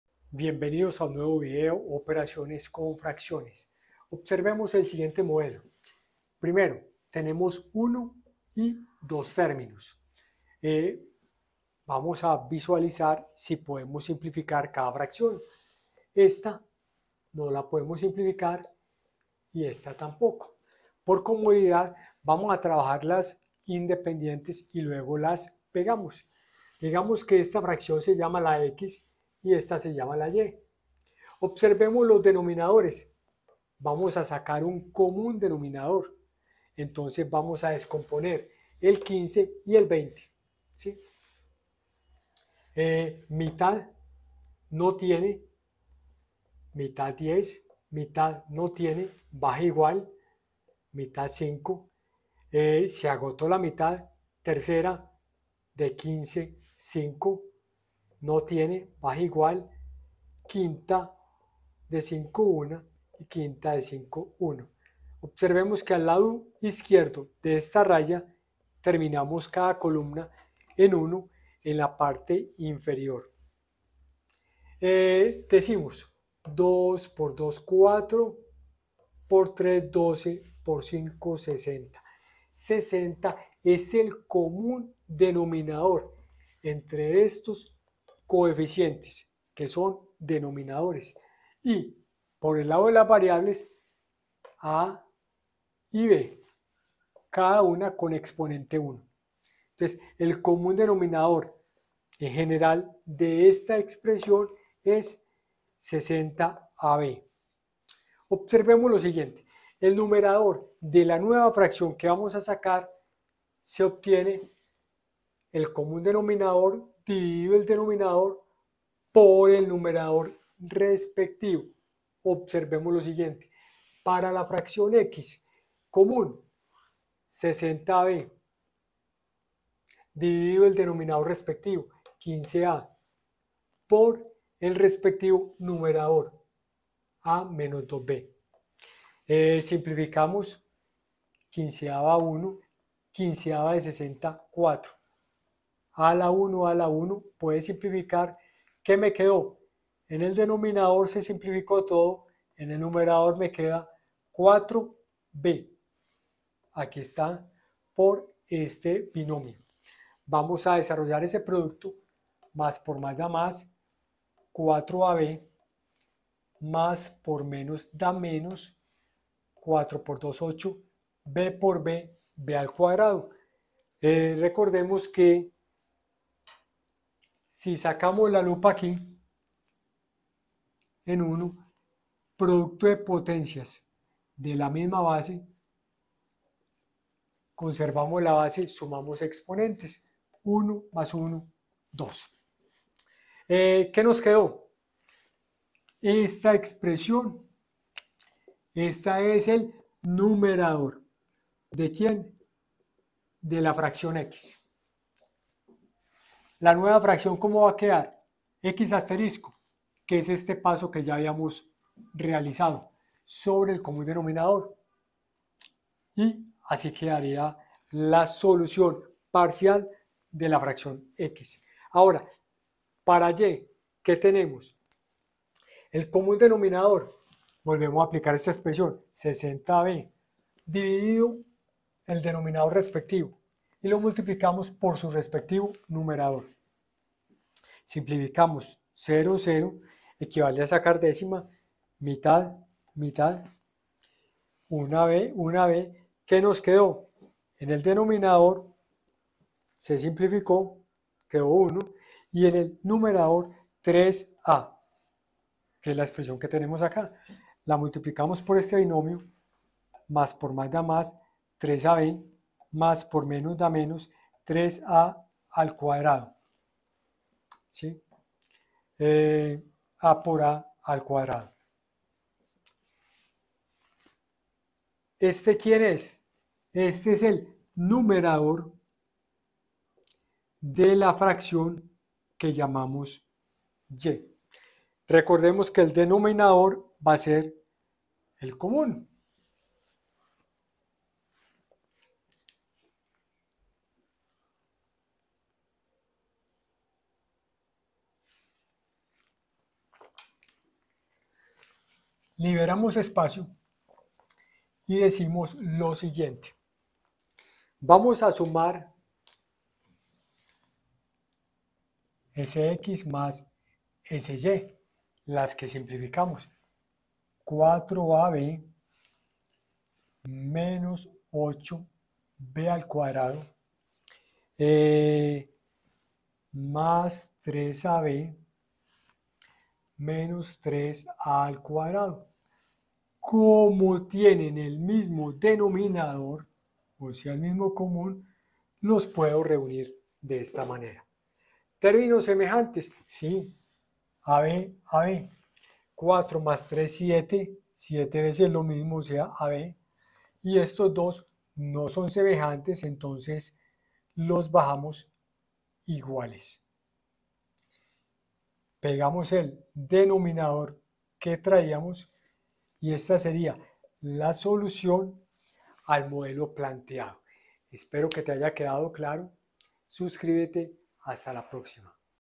Operaciones con fracciones. Video 3.
profenini te explica la operación con fracciones con detalle.